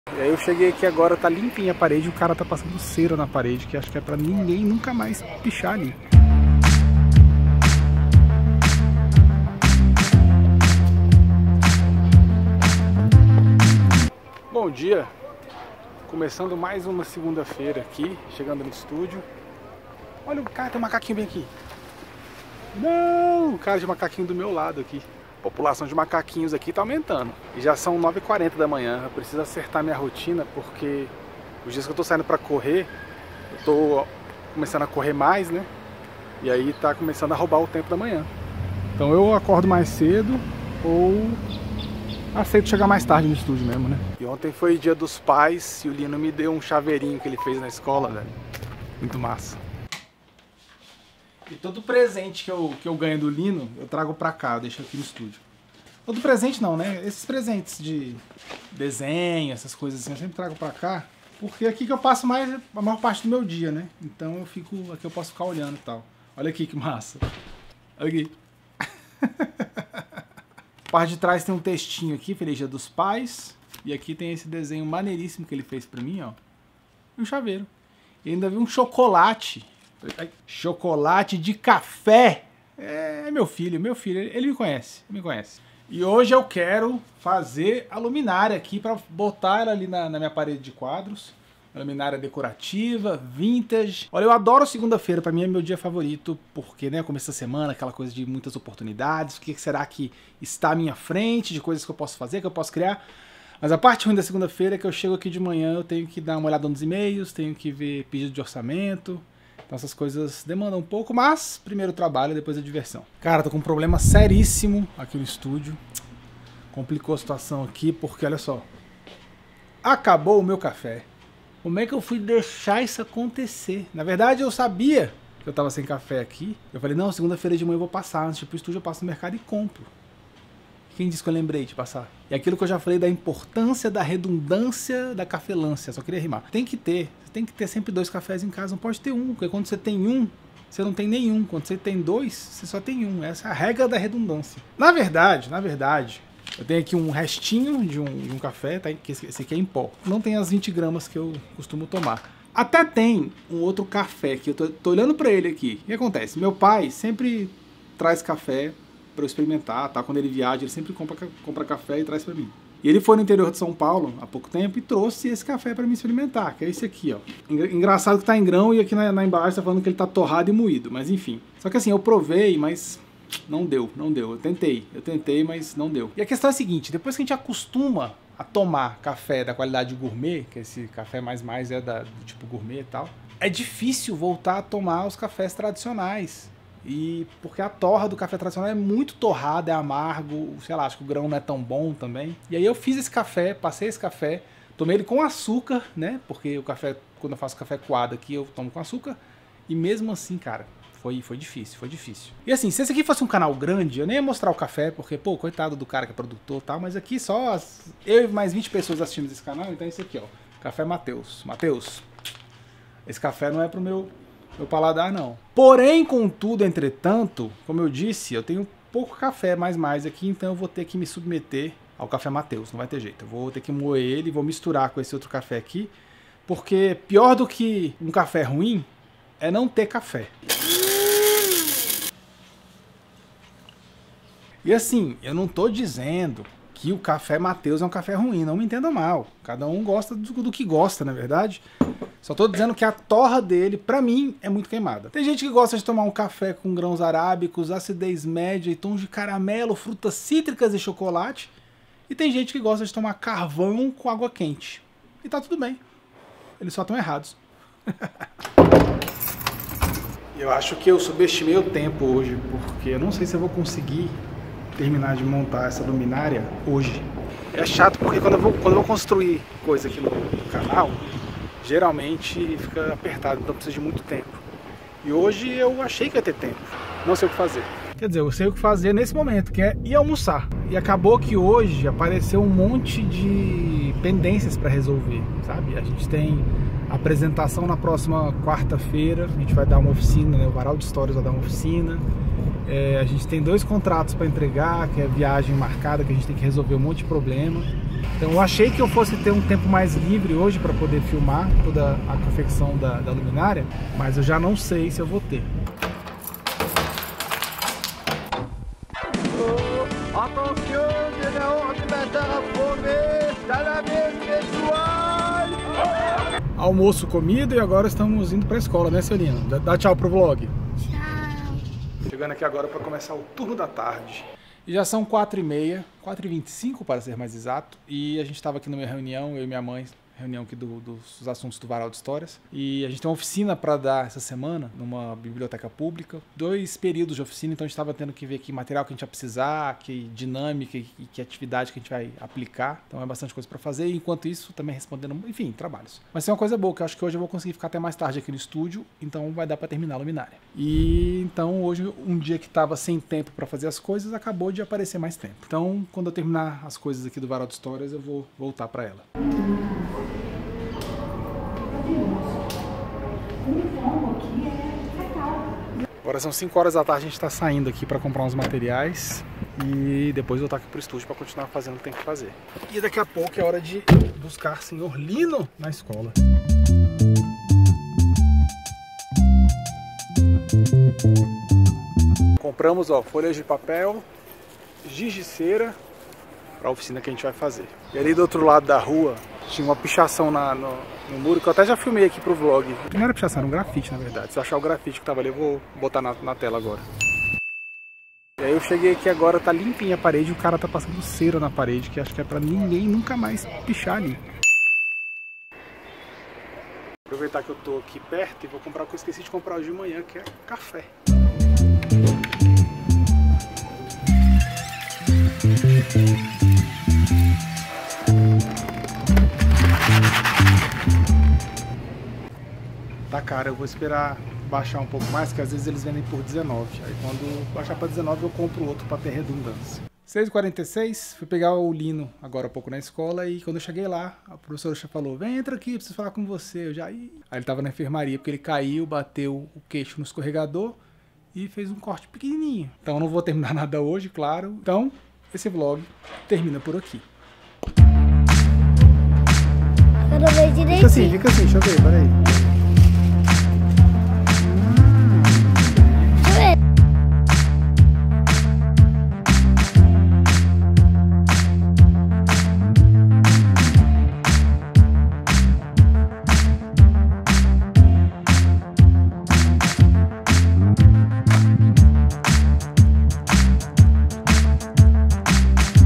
ele apagou todos os grafites da parede | VLOG 73
Hoje eu achei q ia ter tempo pra reformar a luminária vintage mas meu filho acabou se machucando na escola e o dia acabou mais cedo. Mas tá tudo bem ;)
A trilha sonora desse vlog é minha e vc pode ouvir ela
Equipamento usado:
- Canon 80D
- Tokina 11-16mm 2.8
- Rode VideoMicro
- Samsung S7